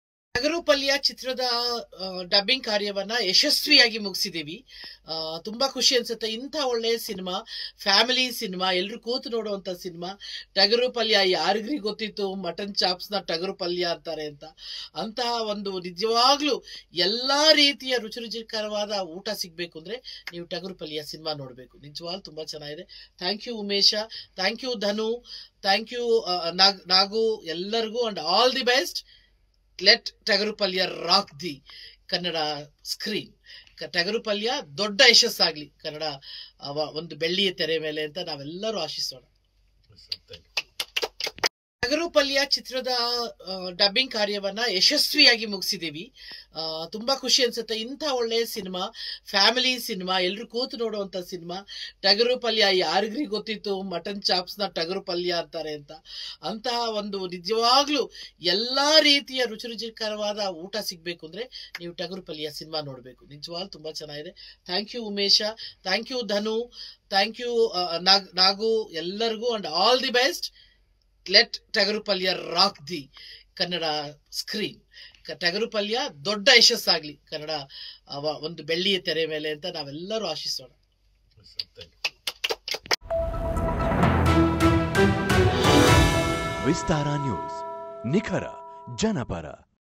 ನಟ ರಾಕ್ಷಸನಿಗೆ ತಾರಮ್ಮ ಥ್ಯಾಂಕ್ಸ್ ಹೇಳಿದ್ದೇಕೆ? | Actor Tara wished Tagarupalya Movie Team |Vistara News
ಸ್ಯಾಂಡಲ್‌ವುಡ್‌‌ನ ಟಗರುಪಲ್ಯ ಸಿನಿಮಾದ ಡಬ್ಬಿಂಗ್ ಮುಗಿದಿದ್ದು, ಚಿತ್ರತಂಡಕ್ಕೆ ನಟಿ ತಾರಾ ಶುಭ ಕೋರಿದ್ದಾರೆ.